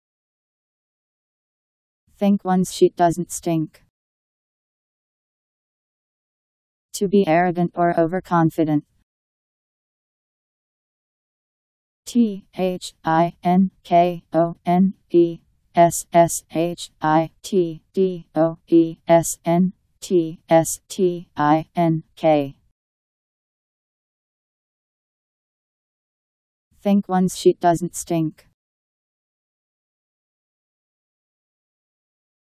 Think one's shit doesn't stink Meaning
Video shows what think one's shit doesn't stink means. To be arrogant or overconfident..  Think one's shit doesn't stink Meaning. How to pronounce, definition audio dictionary. How to say think one's shit doesn't stink. Powered by MaryTTS, Wiktionary